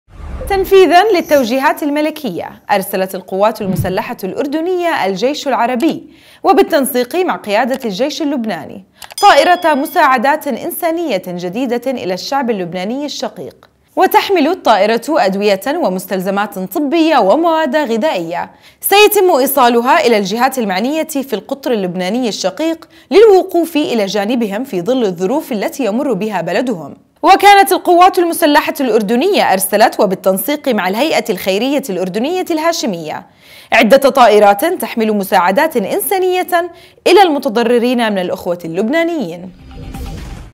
نورسات الأردن : الهيئةُ الخيريةُ الهاشميه ترسلُ طائرةَ مساعداتٍ طبيةٍ جديدةٍ إلى لبنان
تنفيذاً للتوجيهات الملكيه / أرسلت القواتُ المسلحةُ الأردنيه – الجيشُ العربي / وبالتنسيقِ مع قيادةِ الجيشِ اللبناني / طائرةَ مساعداتٍ انسانيةٍ جديدةٍ إلى الشعبِ اللبنانيِ الشقيق / وتحملُ الطائرةُ أدويةً ومستلزماتٍ طبيةً وموادَ غذائيه / سيتم ايصالها إلى الجهاتِ المعنيةِ في القطرِ اللبنانيِ الشقيق للوقوفِ إلى جانبهم في ظلِ الظروفِ التي يمرُ بها بلدُهم / وكانت القواتُ المسلحةُ الأردنيه أرسلت وبالتنسيقِ مع الهيئةِ الخيريةِ الأردنيةِ الهاشميه / عدةَ طائراتٍ تحملُ مساعداتٍ انسانيةً الى المتضررينَ من الأخوة اللبنانيين